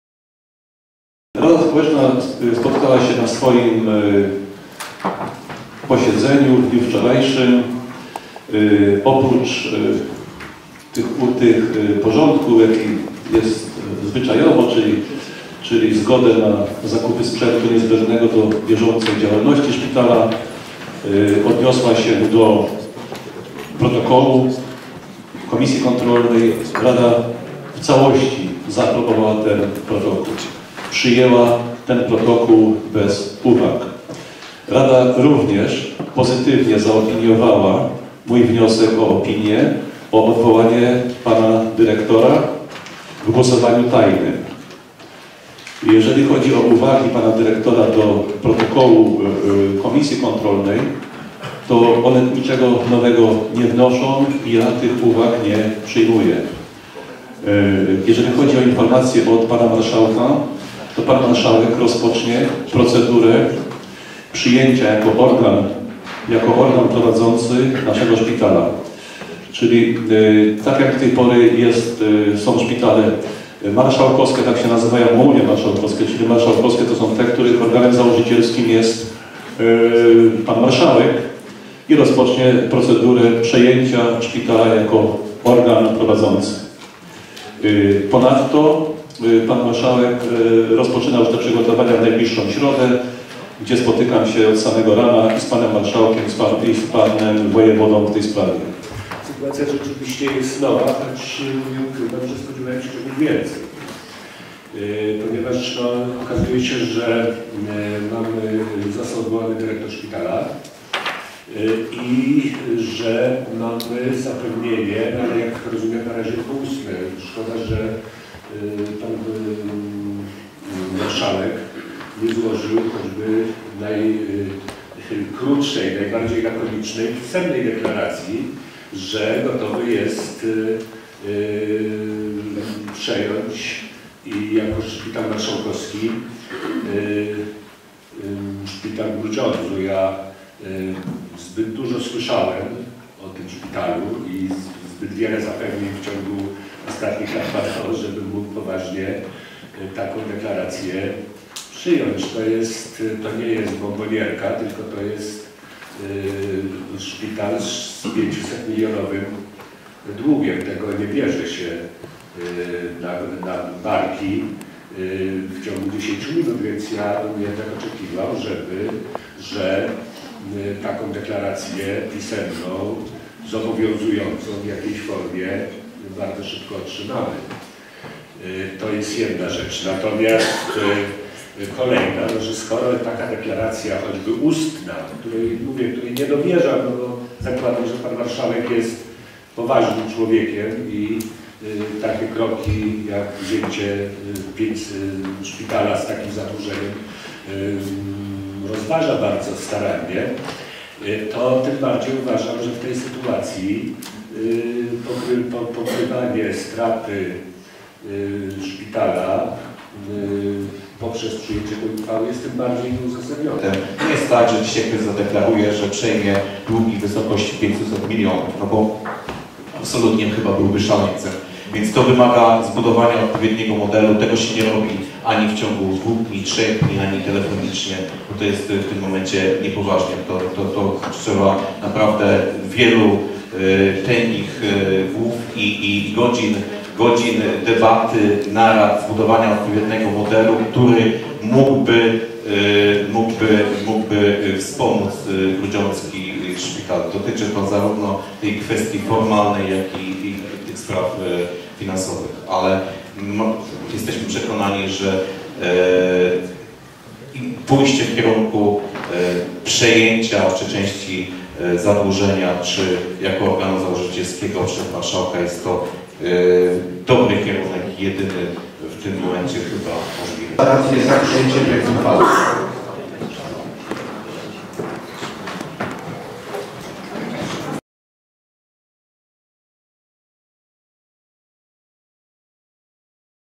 Grudziądzki Szpital przejmie Marszałek województwa?
Grudziądzki Szpital przejmie Marszałek województwa?  Taką deklarację złożył prezydent Robert Malinowski radnym w trakcie sesji.